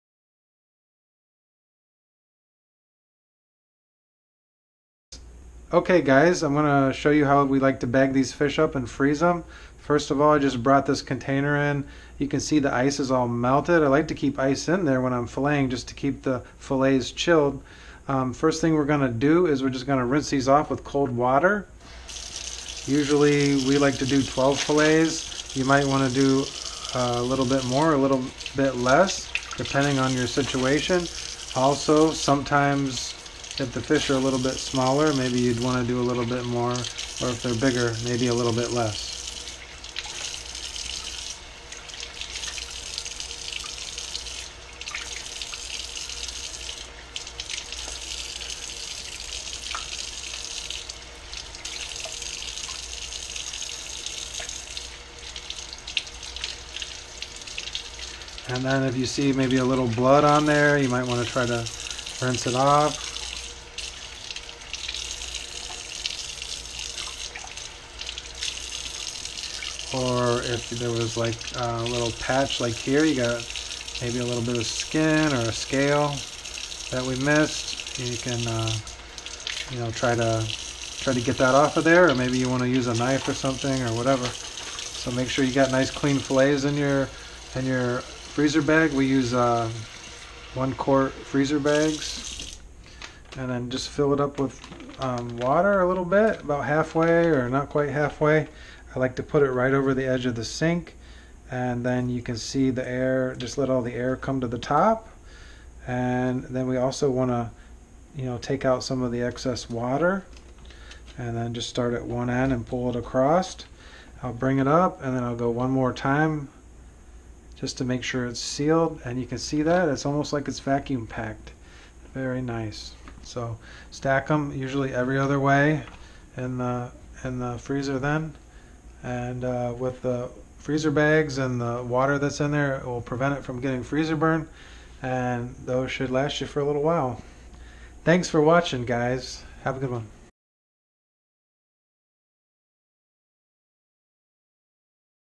How do I freeze Bluegill fillets
Quick video on how to freeze fish.